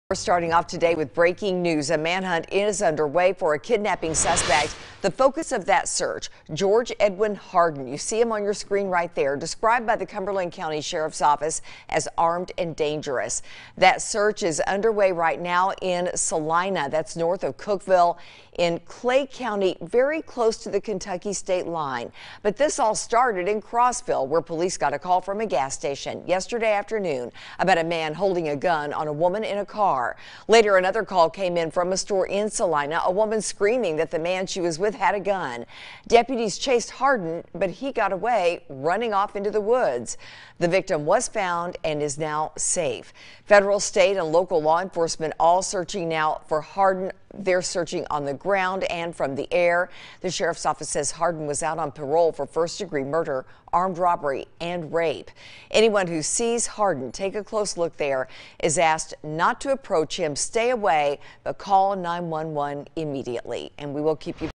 Police search for armed Cumberland County kidnapping suspect
Police are searching for a man described as armed and dangerous who is accused of kidnapping a Cumberland woman at gunpoint.